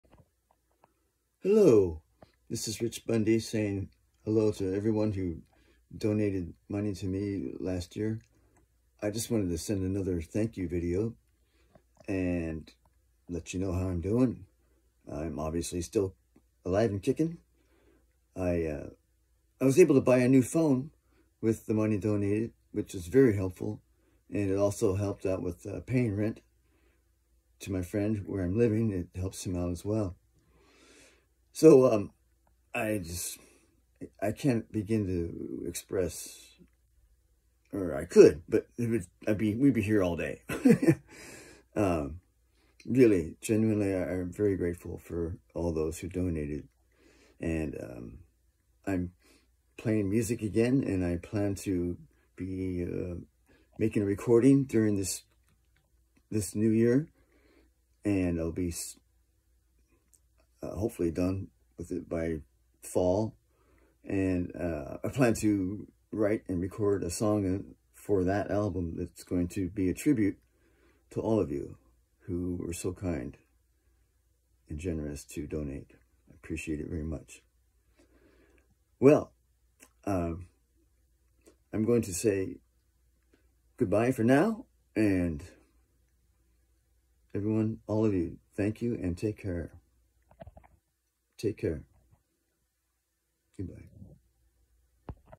An Update from Rich Bundy
An update and thank you video from Rich. While he still hasn't found his own place, he's been able to pay rent for a room in his friend's house thanks to his donors. He was also able to buy a phone with the funds to make recordings and get internet access.

Rich wanted me to pass along his heartfelt thanks to all of you for your kindness and generosity. He has read all your comments and really appreciates your many prayers and good wishes.

If you're interested in helping Rich out, the fundraiser is still happening